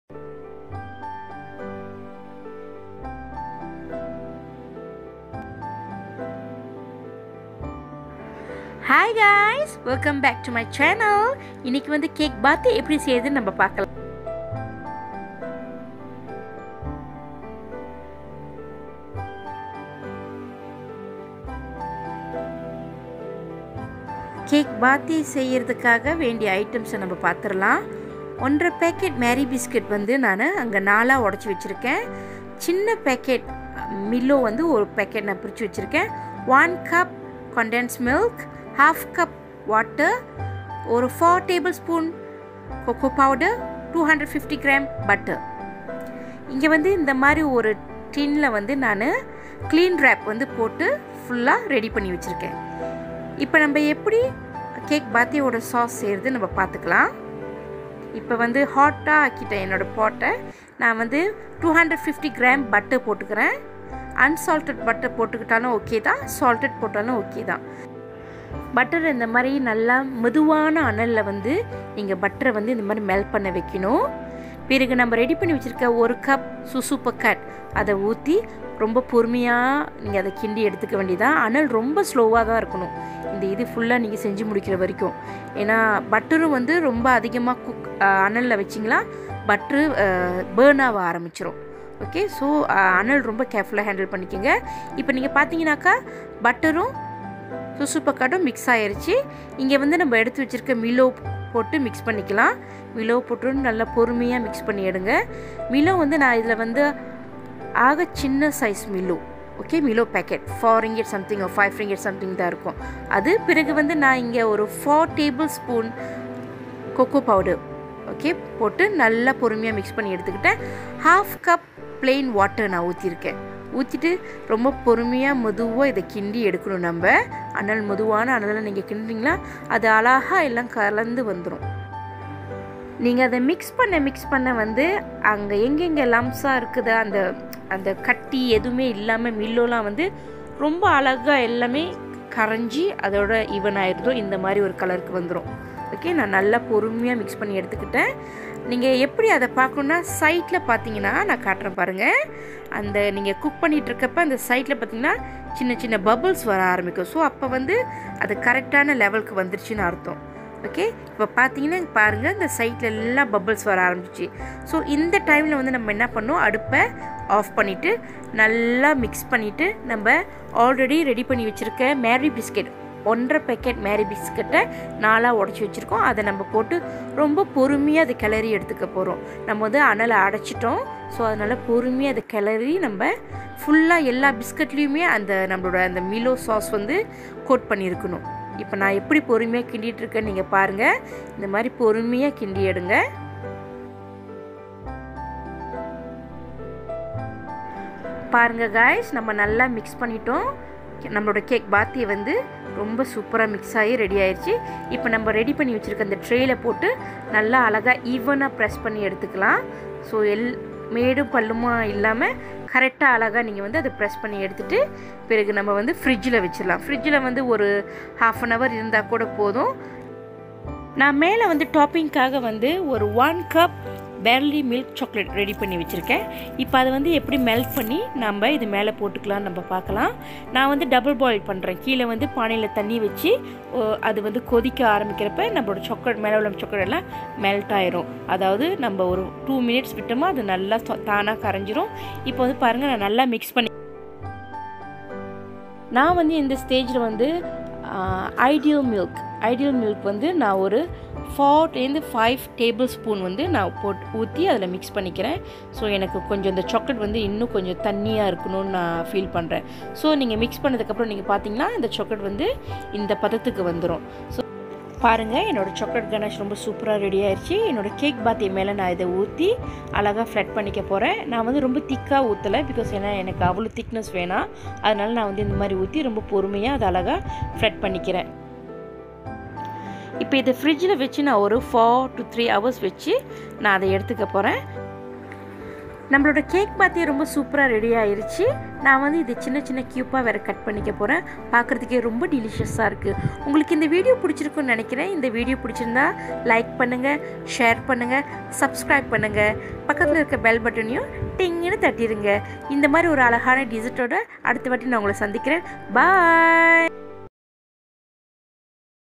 Delicious Kek Batik🤗💕🤤 My kiddos favourite🤩
Bahan - bahannya :👇

- 1 setengah packet biskut marie
- milo packet kecil 1
- 250g  butter
- 1cup susu pekat
- 1/2 cup air
- 4 tbsp coco powder

Topping:👇
- berly milk chocolate 1cup
- 4 tbsp susu cair ideal

selamat mencuba guys🤗